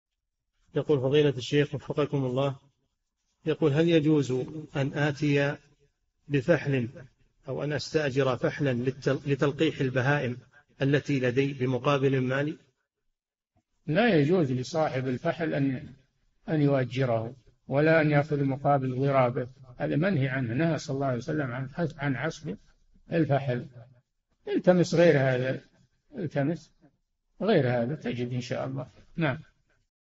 [4040- 4717] هل يجوز لي أن أستأجر فحلا لتلقيح البهائم بمقابل مالي؟ - الشيخ صالح الفوزان
"[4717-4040] هل يجوز أن آتي بفحل أو أن استأجر فحلًا لتلقيح البهائم التي لدي بمقابل مالي؟
الجَوَابُ: لا يجوز لصاحب الفحل أن يؤجره ولا أن يأخذ مقابل ضرابه، هذا منهي عنه، نهى ﷺ عن عسف الفحل، التمس غير هذا، وتجد إن شاء الله!

للاشتراك في قناة الشيخ صالح الفوزان
فتاوى المجالس في تفسير المفصل

للاستفسار والإقتراحات والدعم
رئيس الفريق
لمشروع كبار العلماء